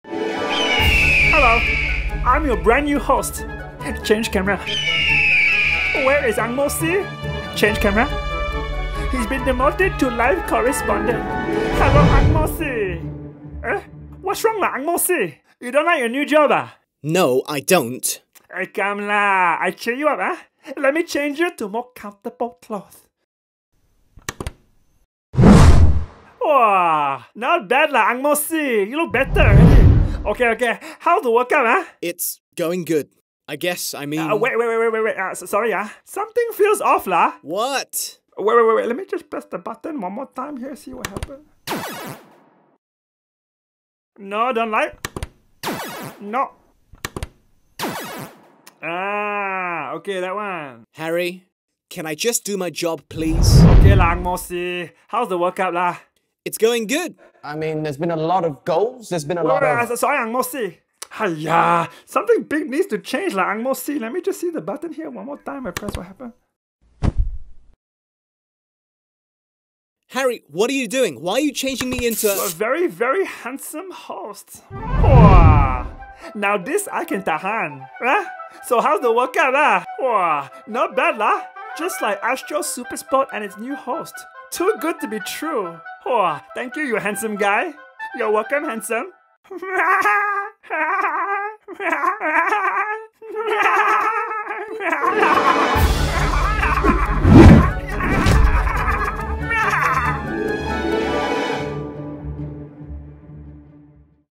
I GOT PROMOTED TO MAIN HOST FOR ASTRO SUPERSPORT!!
I JUST GOT PROMOTED TO MAIN ANCHOR HOST FOR ASTRO SUPERSPORT. ANGMO C, NOT HAPPY!

XO
HARRY